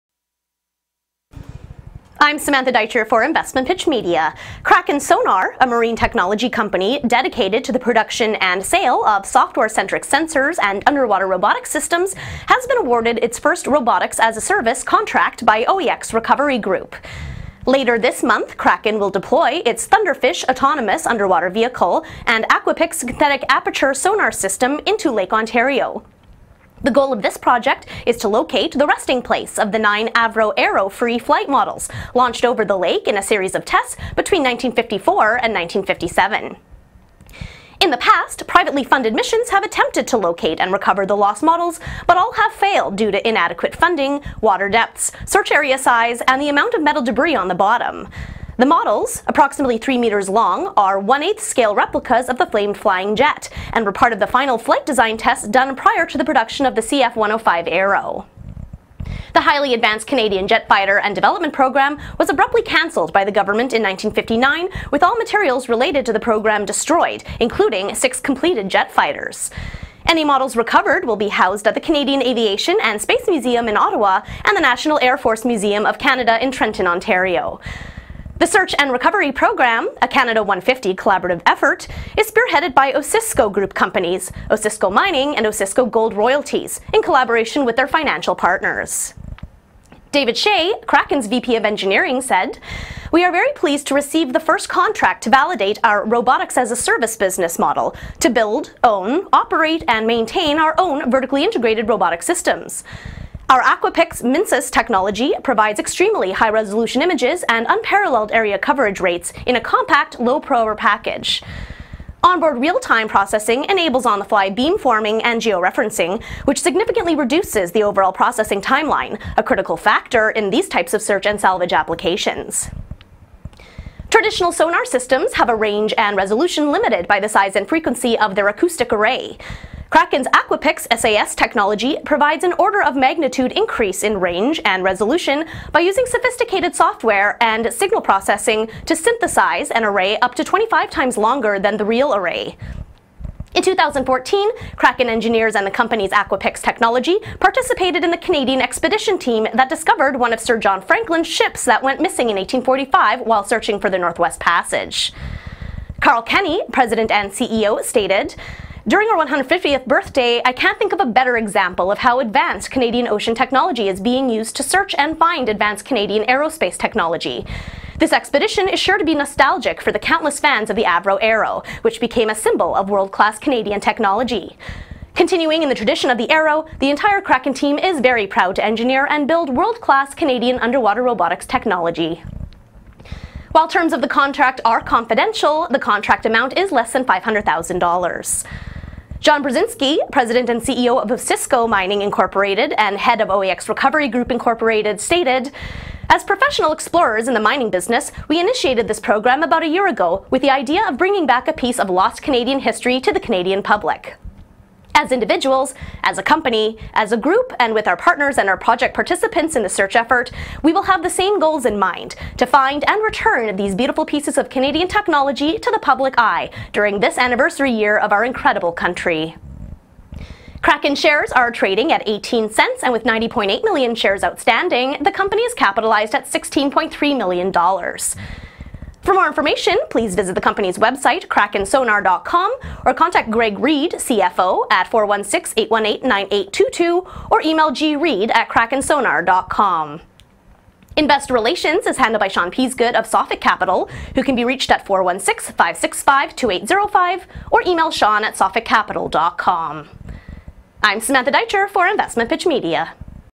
Kraken Sonar (TSXV: PNG) Awarded its 1st "Robotics as a Service" contract
Kraken Sonar, a marine technology company dedicated to the production and sale of software-centric sensors and underwater robotic systems, has been awarded its first “Robotics as a Service” contract by OEX Recovery Group.